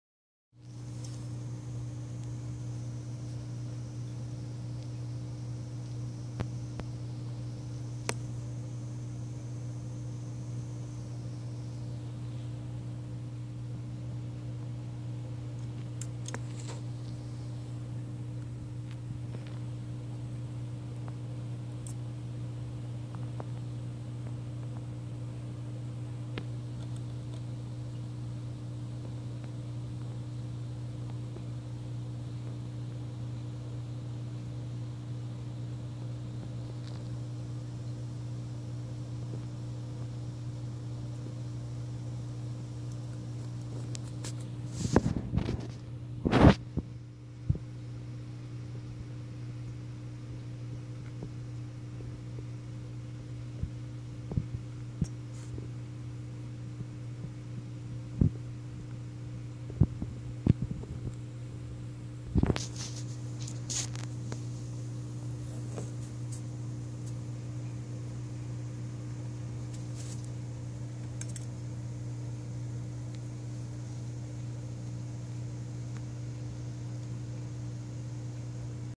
Guinea Pig cuteness
Old video of my Guinea Pig when he had an ear infection.